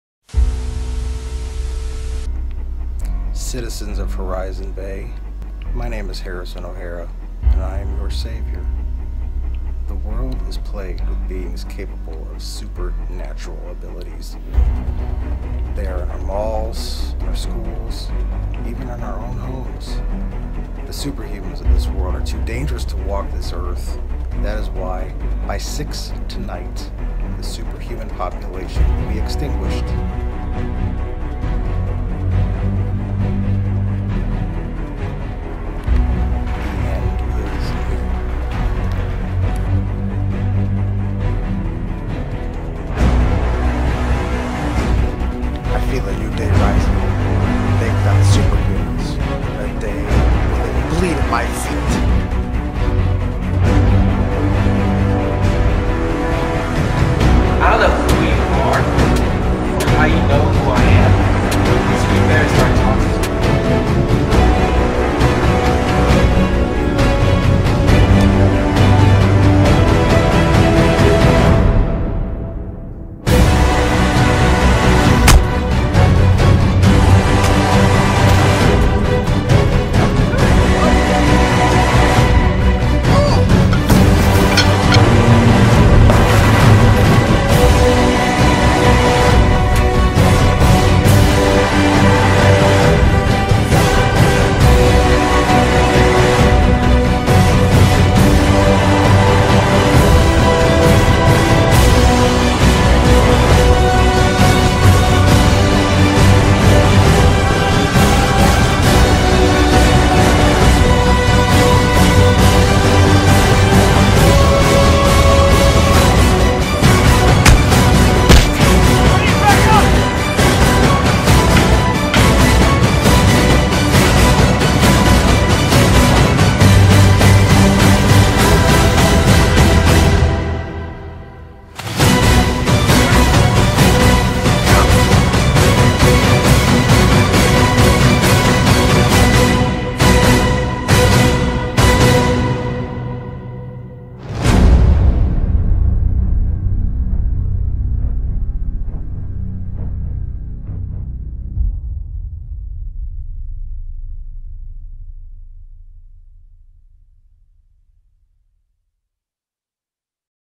Kram's Comics: Nova Strike [Launch Trailer]
With the premiere of Nova-Strike taking place at a private event, YouTube was unfortunately unable to witness the epicness, that is why tomorrow June 18th 2018 the world can! Make sure you turn on your notifications to be the first to see Kram's Comics: Nova-Strike (Director's Cut)!

We really appreciate you taking time out of your day to watch our videos.